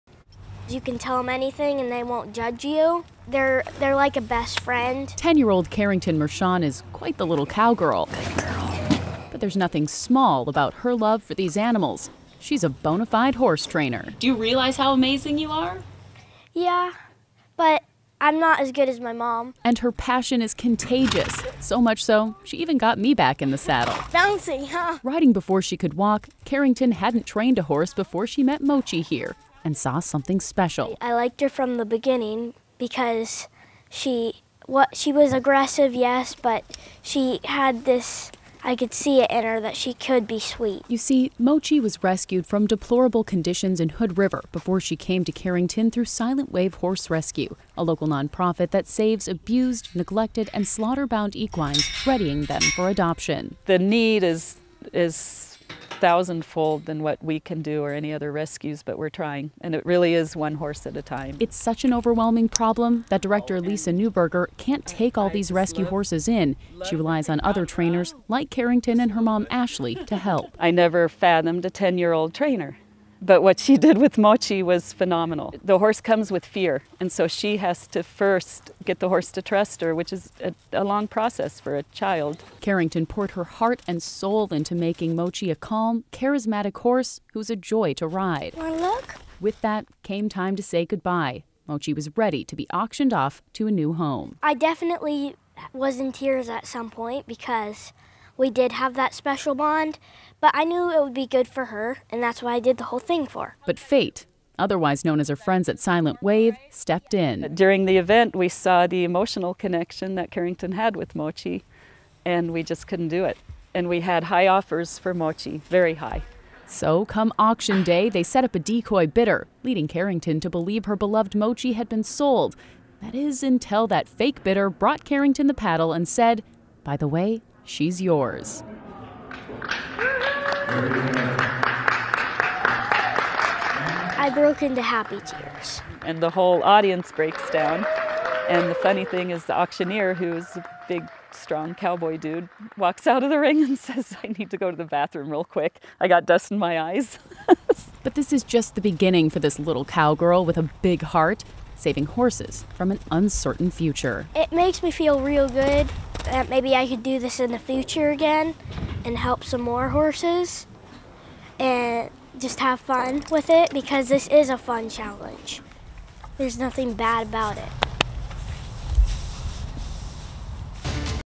10 Year Old Horse Trainer Works With Abused Horse To Give Her A New Life
10 Year Old Horse Trainer Works With Abused Horse To Give Her A New Life